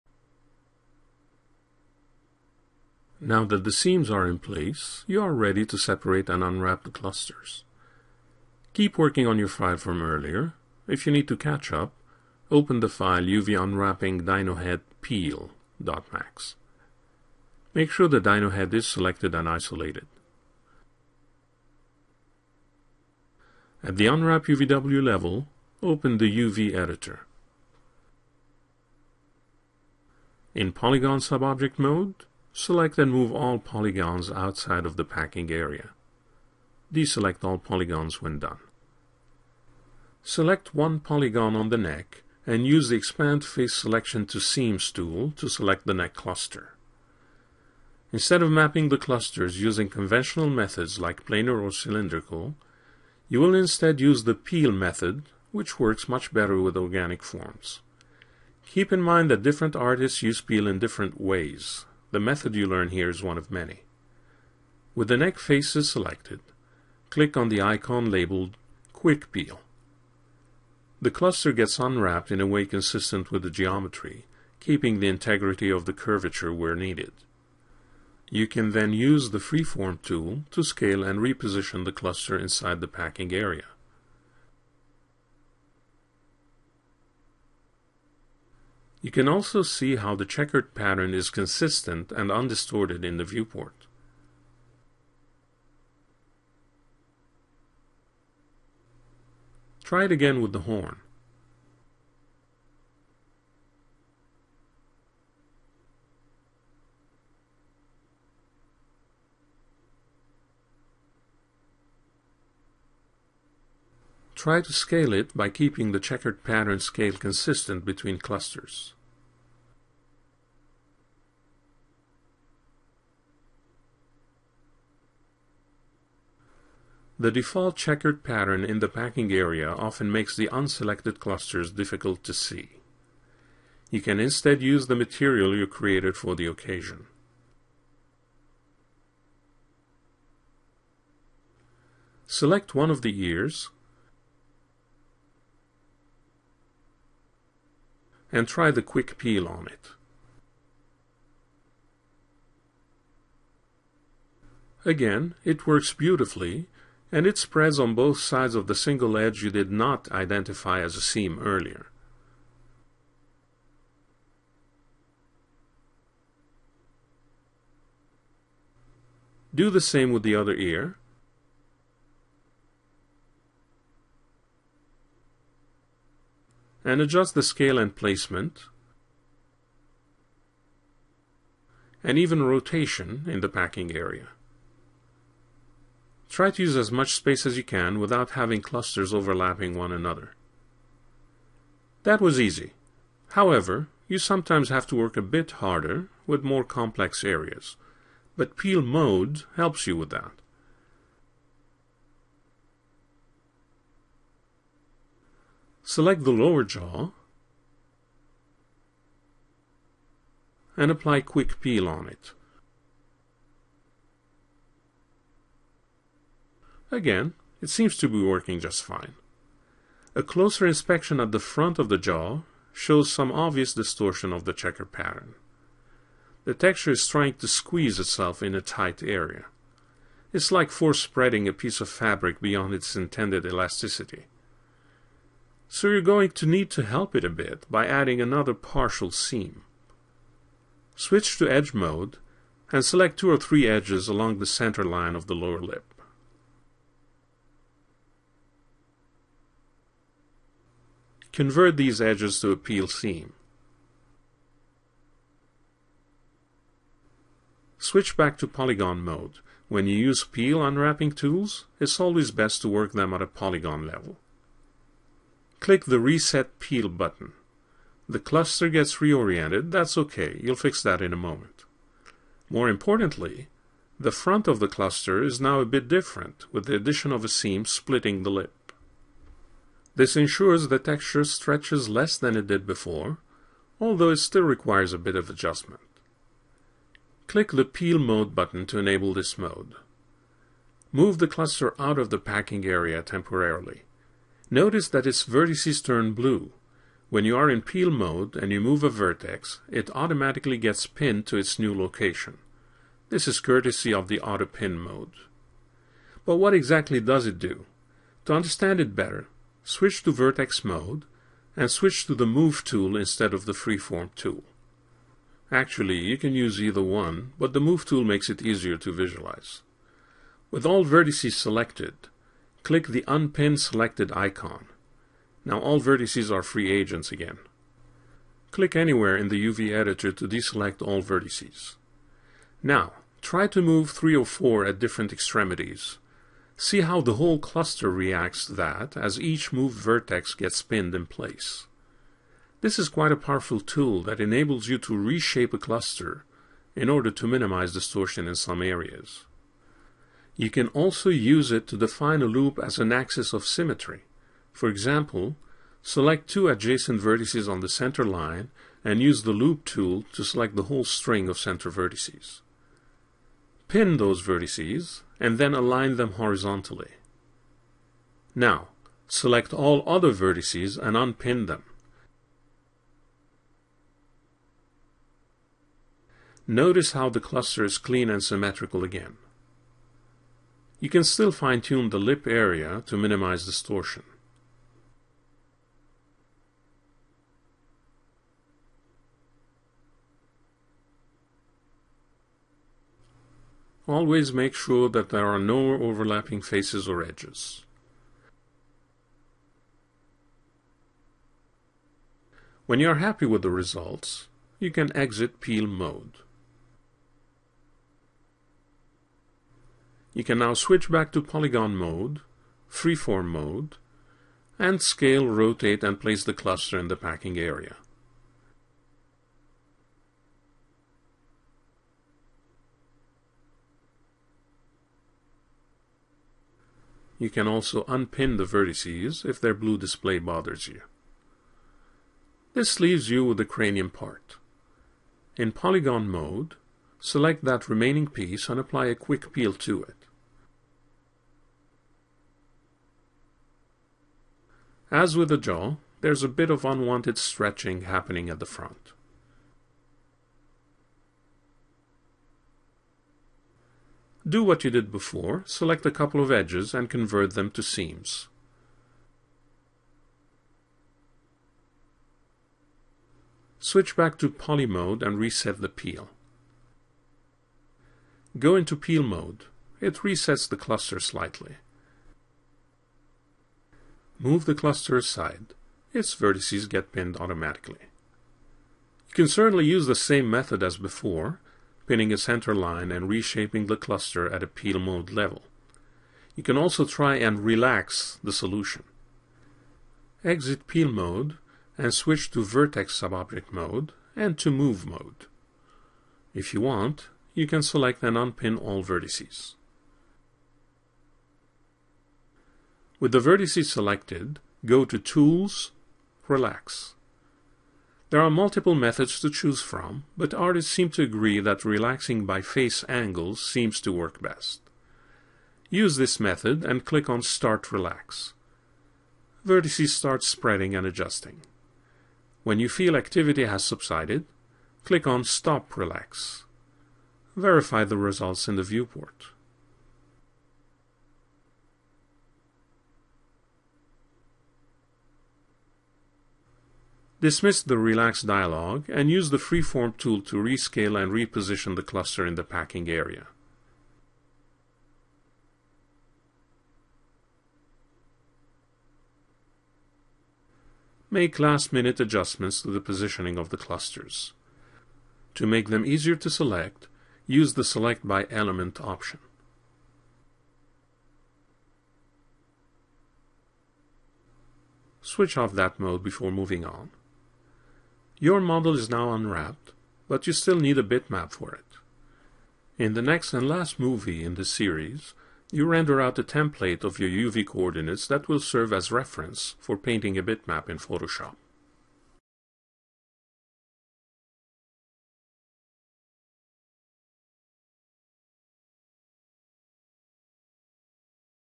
UV Unwrapping in 3ds Max- Part 9 - Organic Model (Cont'd)
In this tutorial, we will continue working on the dinosaur head by unwrapping the neck, ears and horn.
Level: Intermediate
Recorded in: 3ds Max 2013